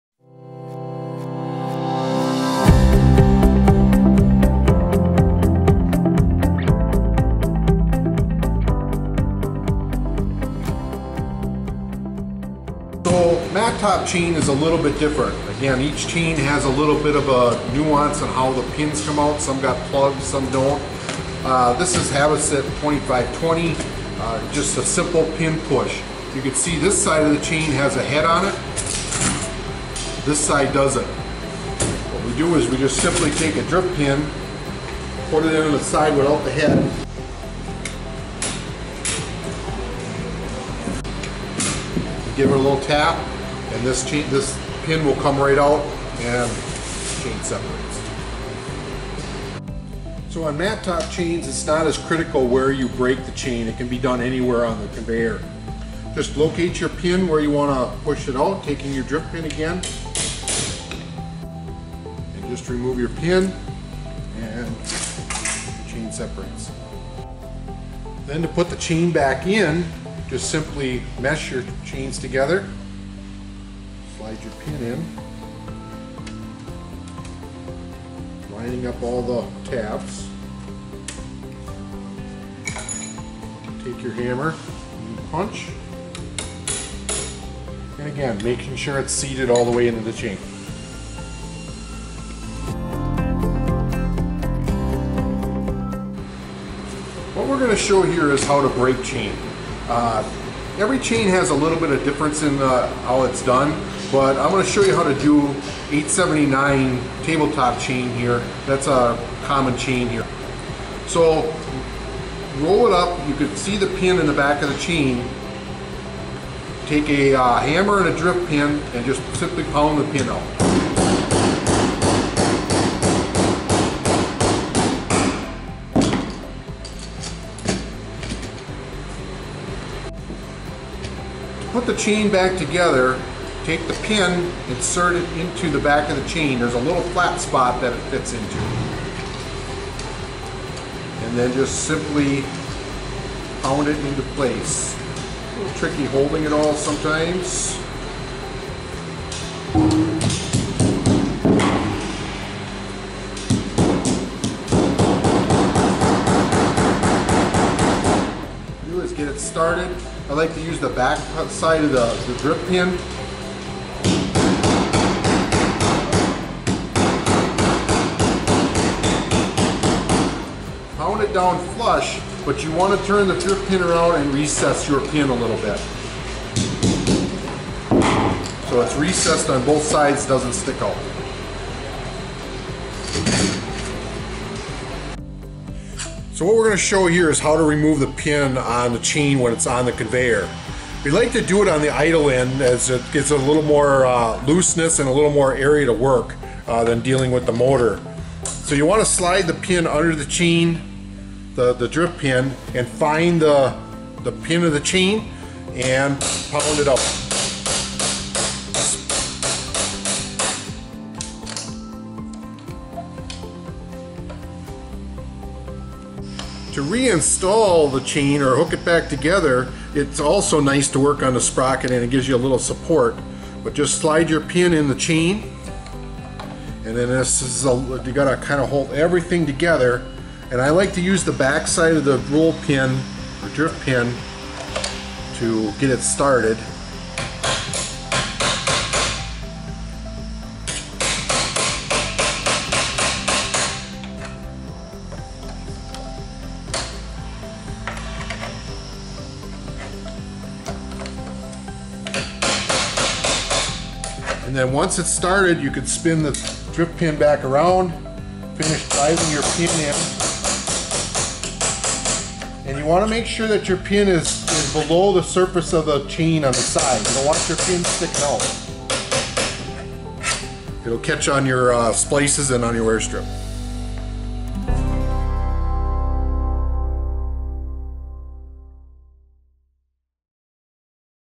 Conveyor Chain Pin Removal and Installation
Nercon's "how-to" on removing and installing mattop and tabletop conveyor chain pins.